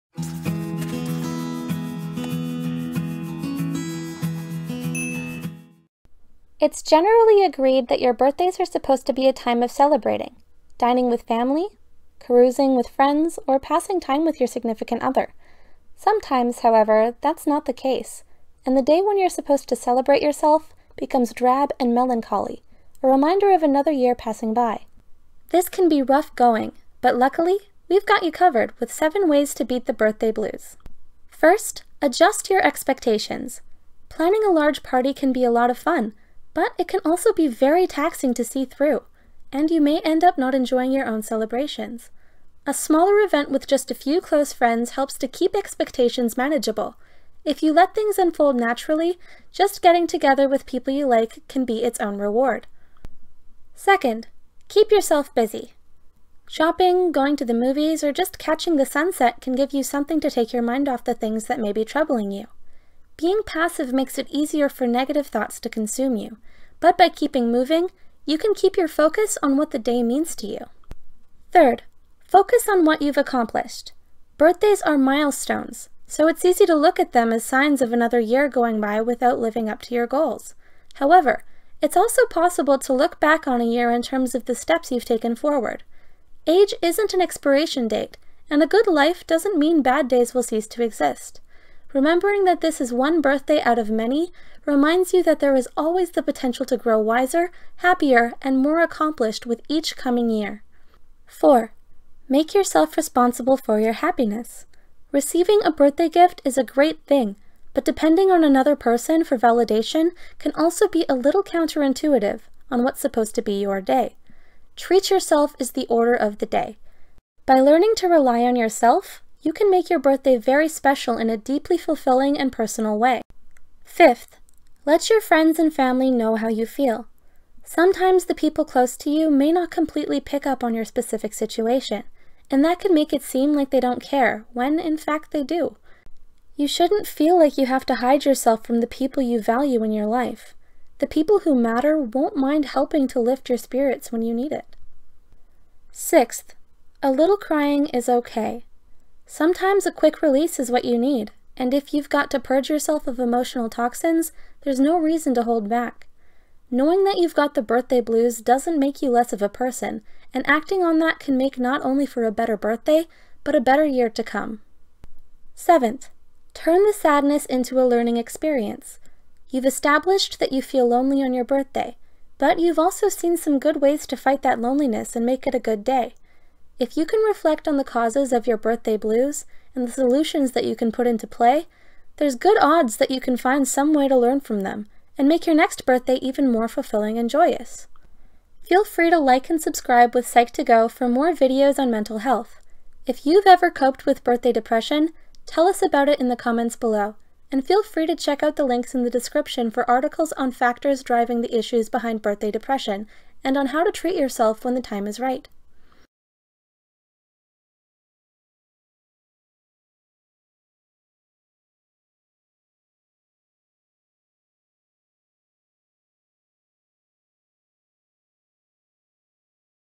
7 Ways To cope With Birthday Depression
Have you heard of the birthday blues? Birthdays often look like having dinner with family and friends at your favorite restaurant or doing something special with your romantic partner. You’re supposed to celebrate you —a time of happiness, success, and fulfillment. So, why does that feel so far away from the truth? Instead, you find yourself locked in your room, shutting the world out, as you blast music with sad lyrics.

It’s hard enough as it already is admitting that you’re not happy as another year already passes, but don’t beat yourself up over it. In this video, we're going to share with you a couple of ways to cope with birthday depression!

Credits:
Writer: Jake Clark
Voice: Star Martin
Animation: Elizabeth Grace